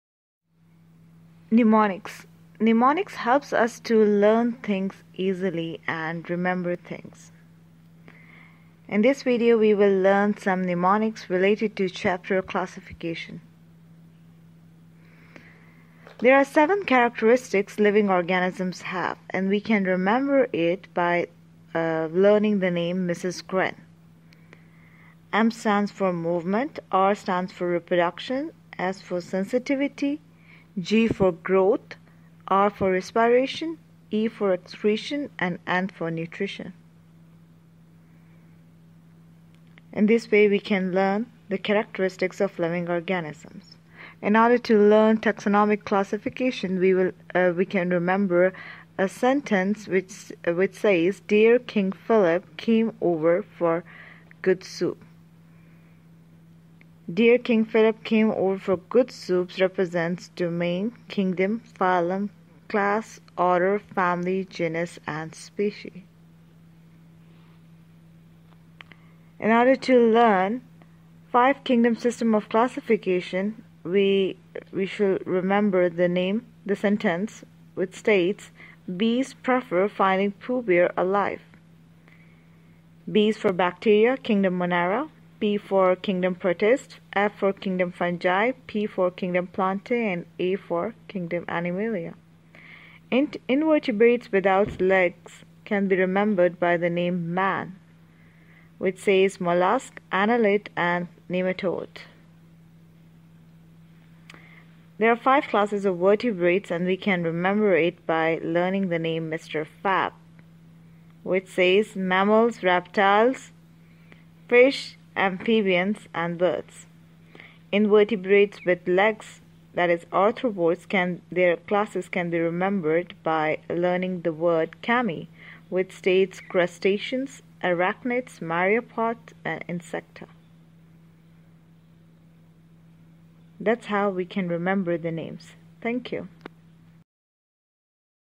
Easy tricks to learn Classification chapter | Mnemonics in biology
Learn Classification chapter with Mnemonics. Easy tricks to study Classification chapter.